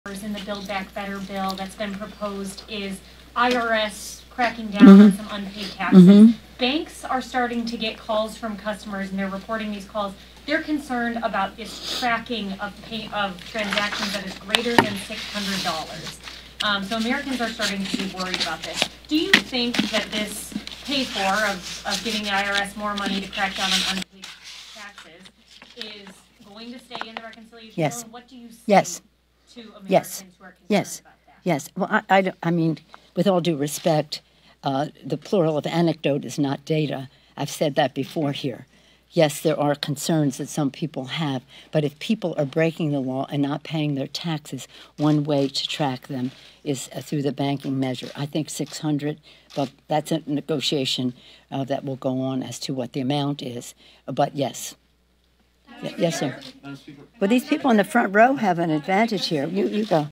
Pelosi Adamant That Extra IRS Funding Will Remain In Reconciliation Bill
Speaker of the House Nancy Pelosi holds a press conference about the reconciliation bill and debt limit.